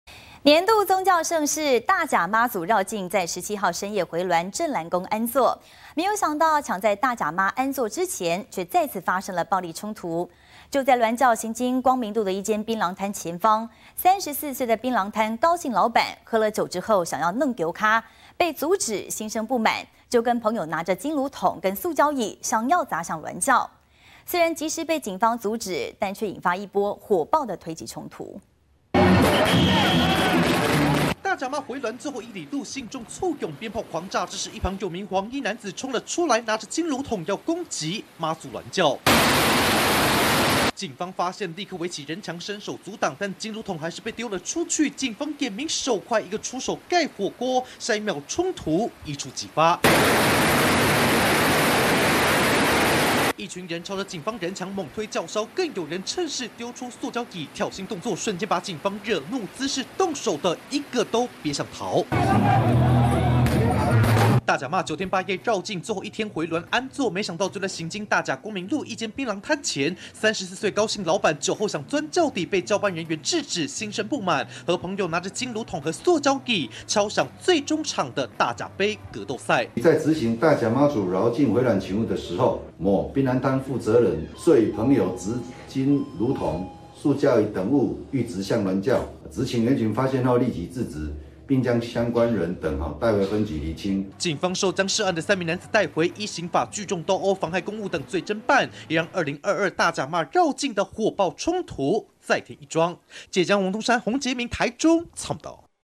大甲盃格鬥賽又開打 竟持金爐桶扔鑾轎
年度宗教盛事-大甲媽祖遶境，17號深夜回鑾安座，沒想到搶在大甲媽安座前，再發暴力衝突，就在鑾轎行經光明路一間檳榔攤前，34歲的檳榔攤高姓老闆，酒後想要鑽轎底被制止，心生不滿，與朋友持金爐桶和塑膠椅要砸向鑾轎，雖然及時被警方制止，卻引發一波火爆的推擠衝突！